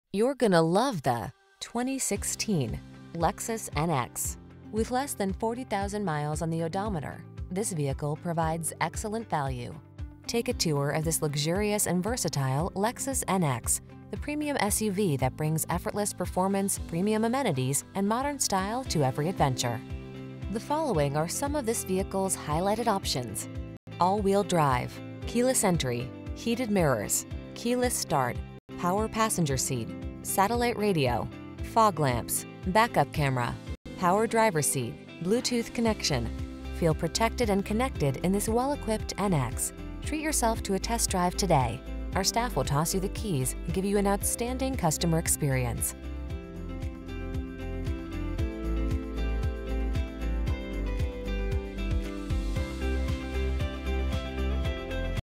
2016 Lexus NX Rochester, Victor, Pittsford, Webster, Spencerport, NY VT45203
White Used 2016 Lexus NX 200t F Sport available in Rochester, New York at Auction Direct USA Rochester. Servicing the Victor, Pittsford, Webster, Spencerport, NY area.

AWD! Navigation/GPS, Moonroof/Sunroof, Bluetooth, Backup Camera, Leather Interior, Heated Steering Wheel, Heated Seats, Dual Power Seats, Power Liftgate, Remote Keyless Entry, Push Button Start, Paddle Shifters, Steering Wheel Mounted Audio Controls, USB Port, Alloy Wheels. This vehicle has a clean, accident-free CARFAX history report.